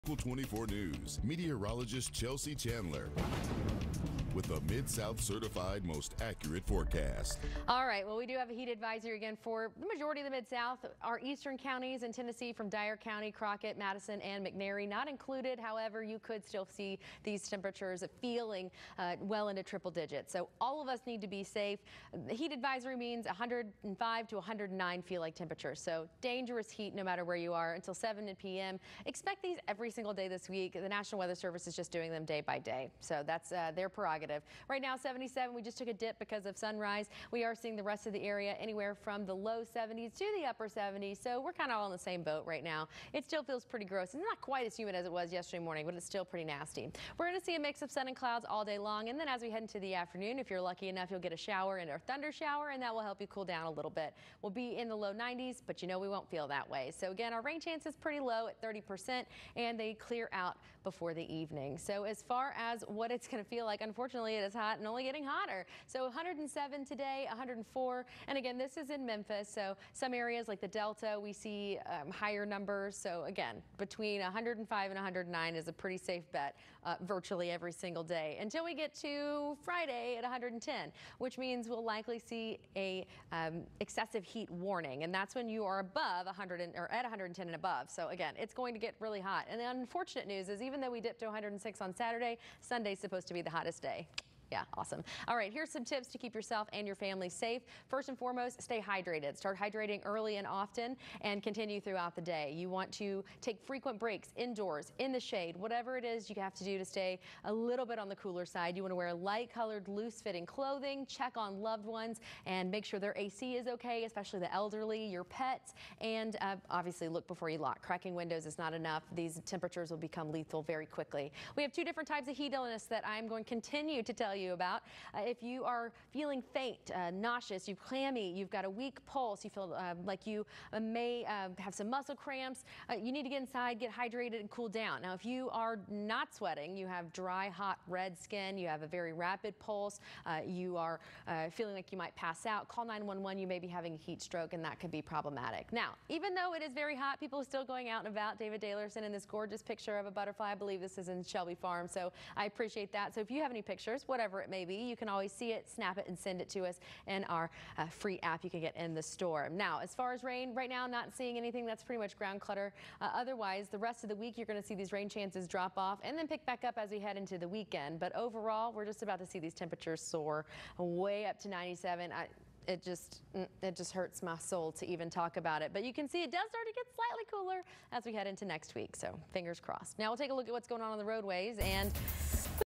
WATCH: Tuesday AM Forecast with Chelsea Chandler 07-27-21
More dangerous heat today and it's only getting hotter.